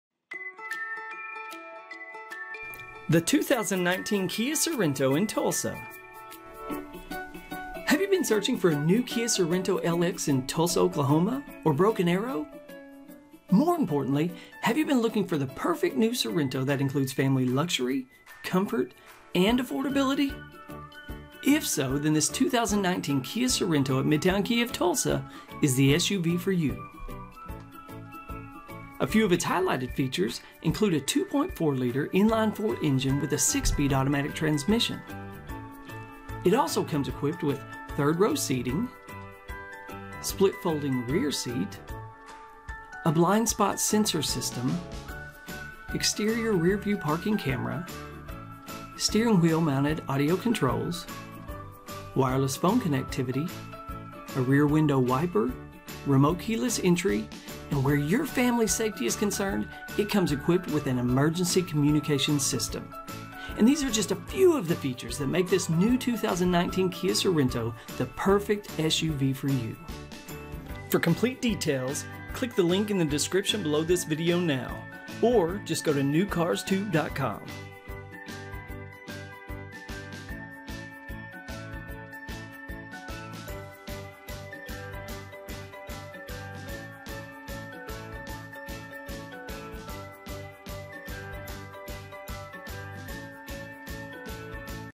2019 KIA Sorento LX AWD in Tulsa, OK | Sparkling Silver | Stock# 10149
2019 KIA Sorento 2.4L LX AWD SUV For Sale in Tulsa, OK!

Vehicle Identification Number (VIN): 5XYPGDA38KG527582
Stock#:  10149
Exterior Color:  Sparkling Silver
Interior Color:  Satin Black

THIS VEHICLE IS LOCATED AT:

Midtown KIA of Tulsa
4747 S. Yale AVE.